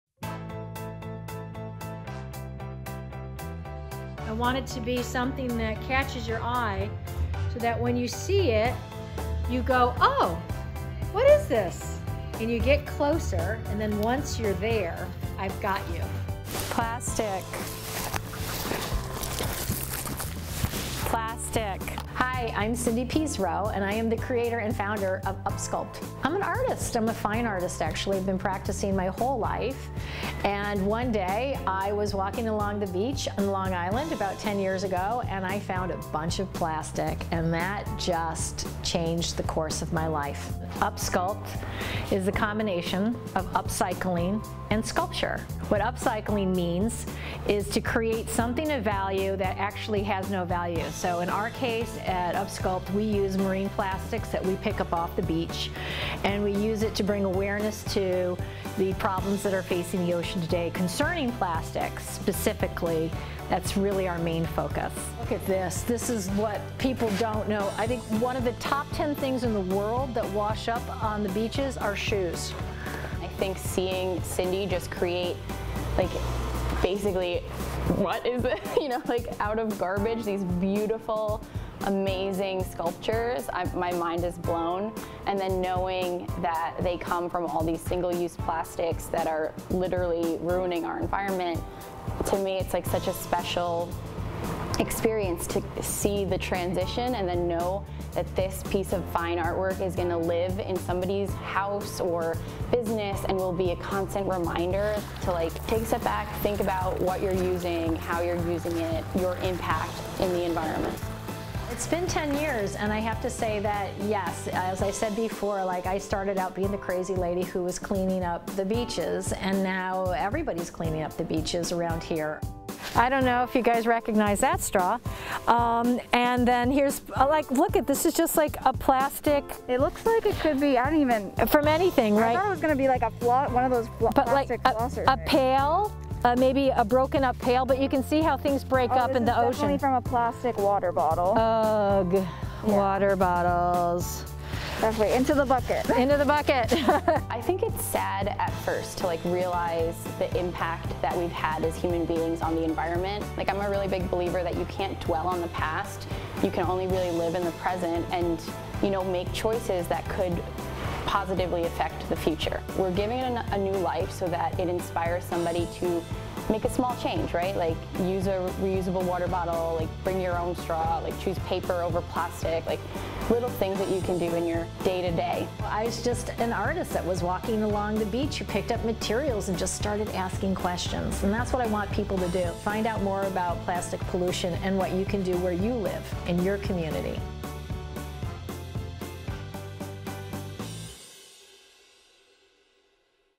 Artist creates beautiful pieces from trash she collects to bring awareness to marine pollution
For as long as Cindy Pease Roe can remember, art has been a part of life.

In addition to her passion for the arts, she loved the outdoors, especially the ocean.

During Roe’s early twenties, she lived on a sailboat and traveled throughout the Caribbean Island’s where she was inspired to create art from the variety of fish she had seen.

This newfound appreciation for marine life became a part of her work and has been ever since.

10 years ago, she was walking her dogs on one of the Long Island beaches and was shocked by all the plastic washed up onshore.

From painting, she pivoted to sculpting, creating art pieces with the trash she would collect from the beaches.

 Now on a mission to stop marine pollution and educate others with art, she created the non-profit, UpSculpt.

“UpSculpt is the combination of upcycling and sculpture,” said Roe. “What upcycling means is to create something of value that actually has no value. In our case, at UpSculpt, we use marine plastics that we pick up off the beach, and we use it to bring awareness to the problems that are facing the oceans today concerning plastics.”

At her art studio in Greenport, instead of paintbrushes and blank canvases, you can find an assortment of cleaned trash.

In her studio, the trash she has collected or donated from people get cleaned and organized by type, color, and shape.

Some of these materials include tampons, bullet-casings, liquor bottles, straws, and even beach sandals.